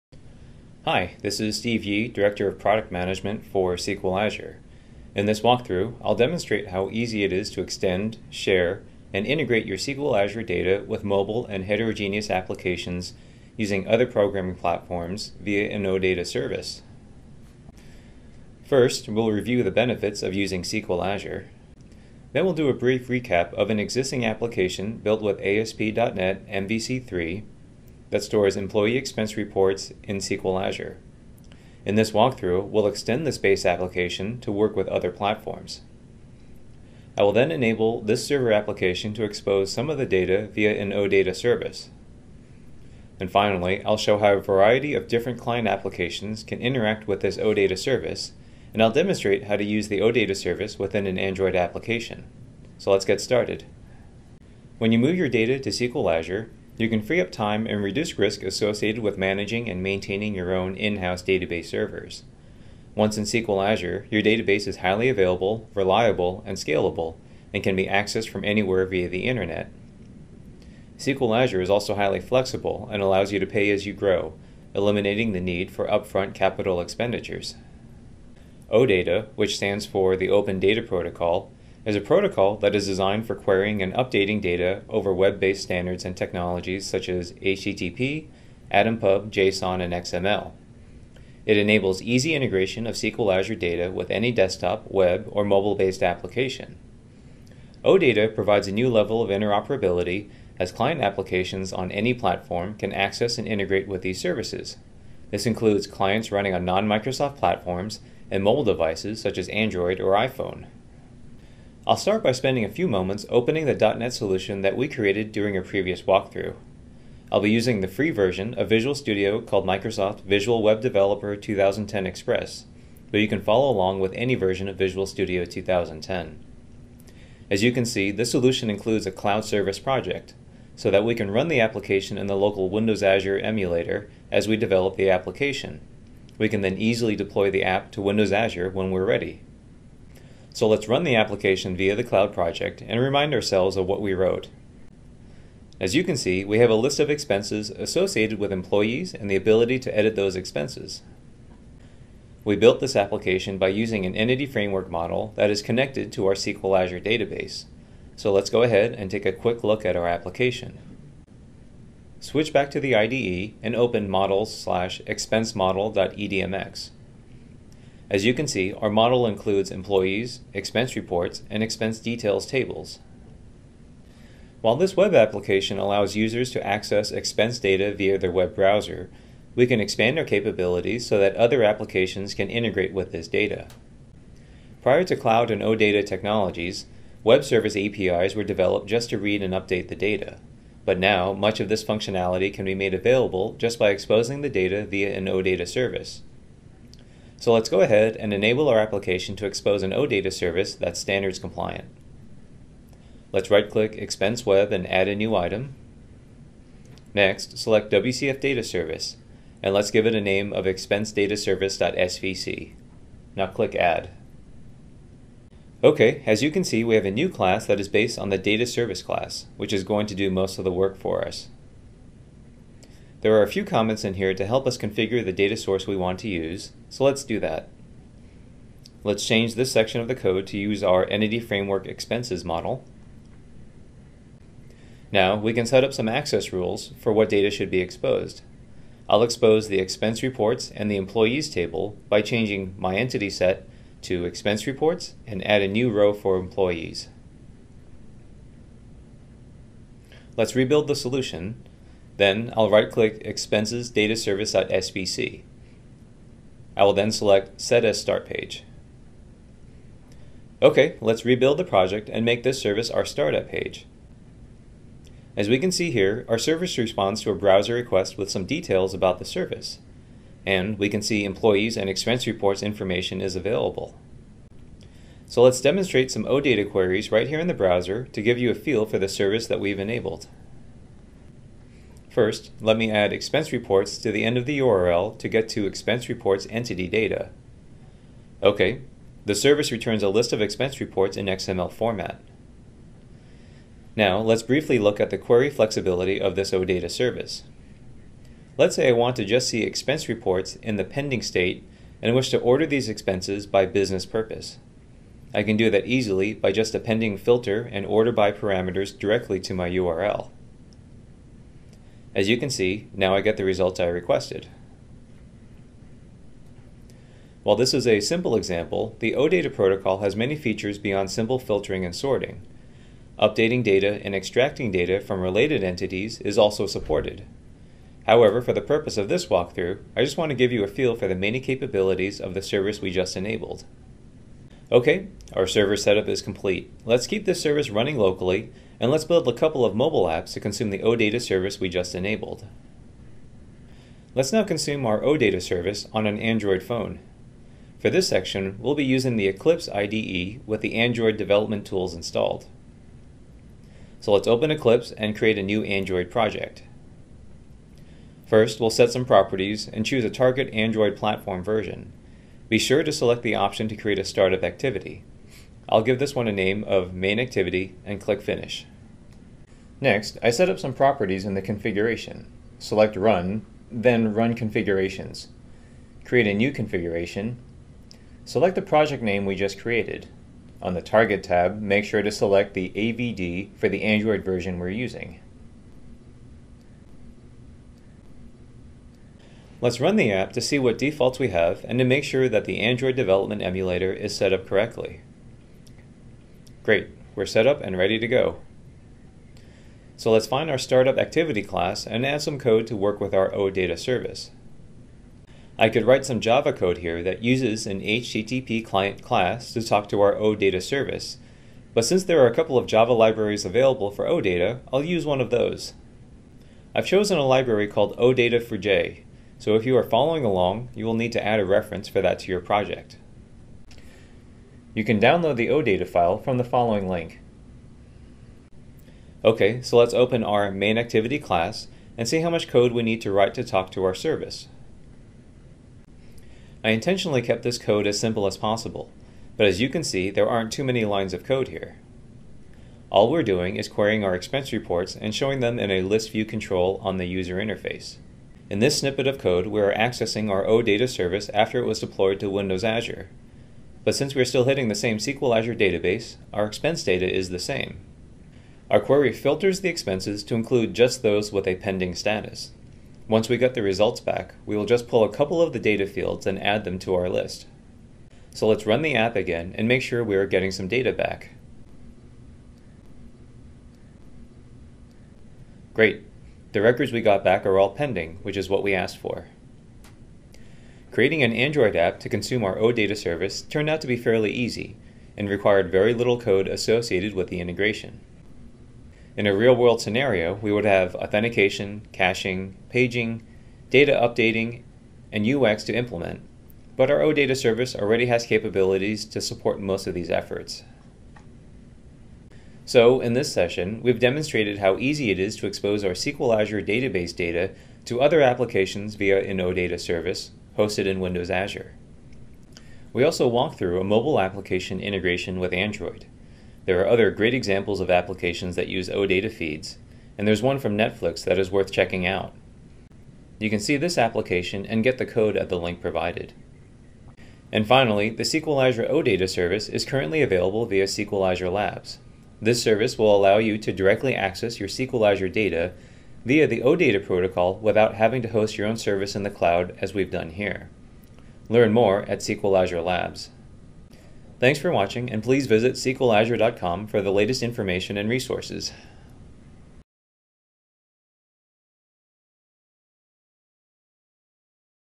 Extending SQL Azure Data to Mobile and Heterogeneous Platforms
This walkthrough demonstrates how easy it is to extend, share, and integrate SQL Azure data with mobile and heterogeneous applications using other programming platforms via an OData service. The video starts by covering the benefits of using SQL Azure, and then it briefly reviews an existing ASP.NET application that was built to support this series of videos. The video goes on to cover how to enable the application to expose some of the data via an OData service, and finally, it shows how a variety of different client applications, such as an Android phone, can interact with this OData service. The video concludes by referring users to additional resources that offer further support.